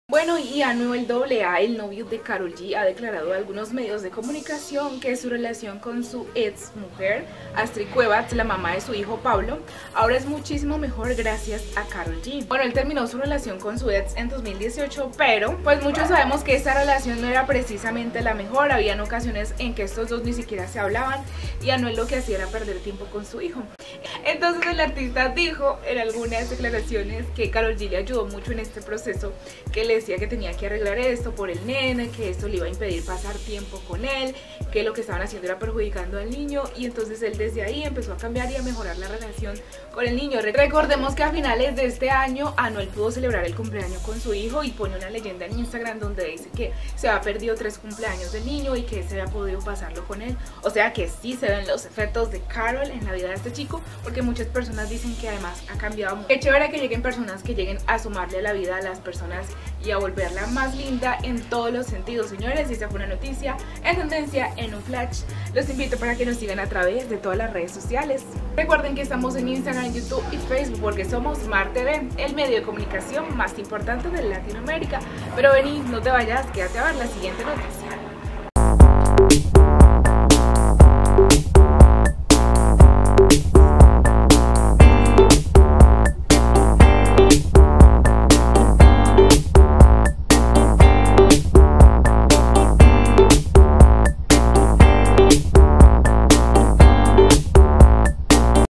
MIRA LO QUE HIZO KAROL G CON LA EX DE ANUEL AA
KAROL G HA HECHO CAMBIOS EN LA VIDA DE ANUEL AA